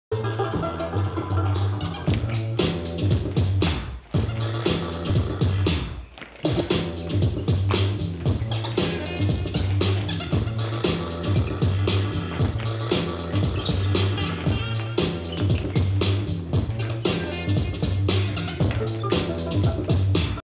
Dance Centre ONLINE / Тренировка Breaking
Танцевальный центр "ONLINE"
Тренировка у группы break-dance
02.07.16 г. Магадан